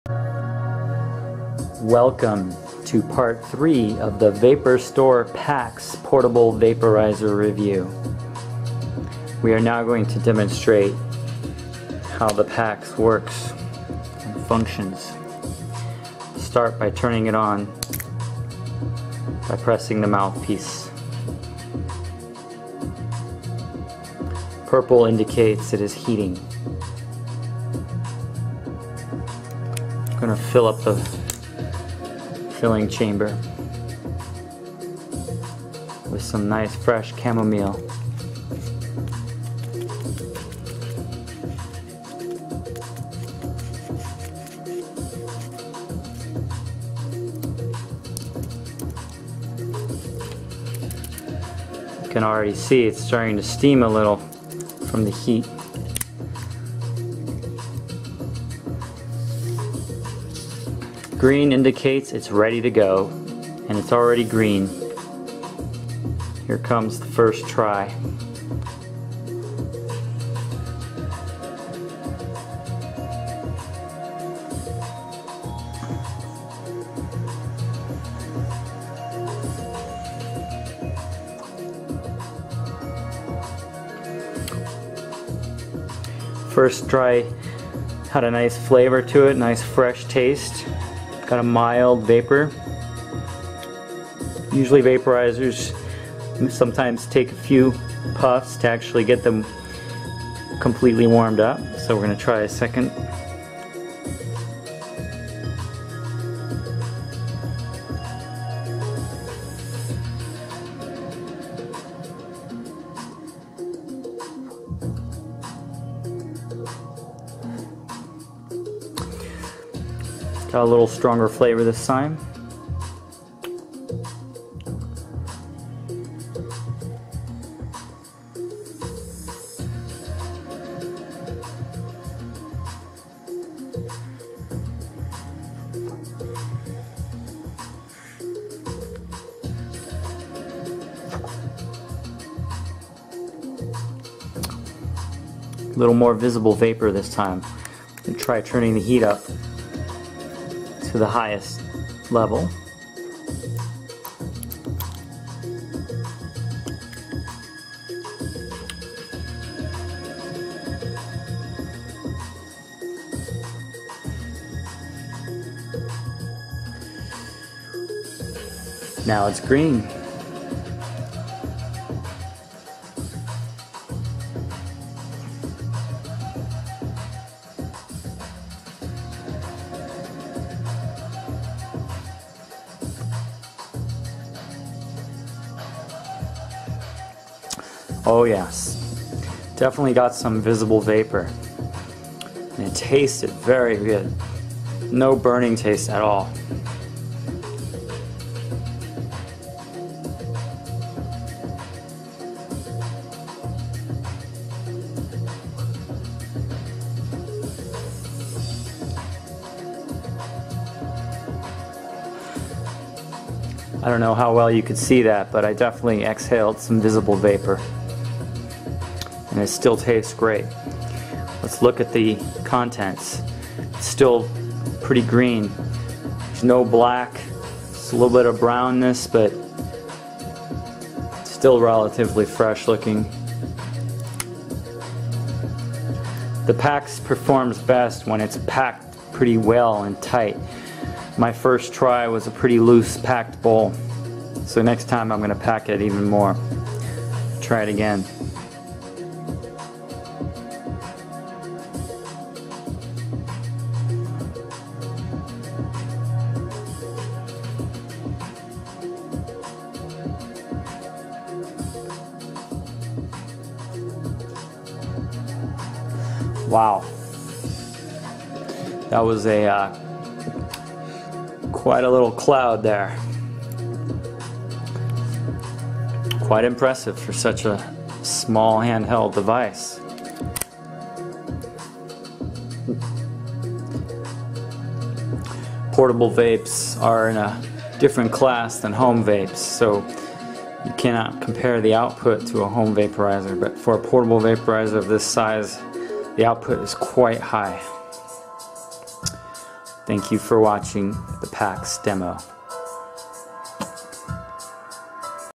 How To Use The PAX Portable Vaporizer By Ploom
In this video we show how to use the PAX Portable Vaporizer by Ploom and all it's features.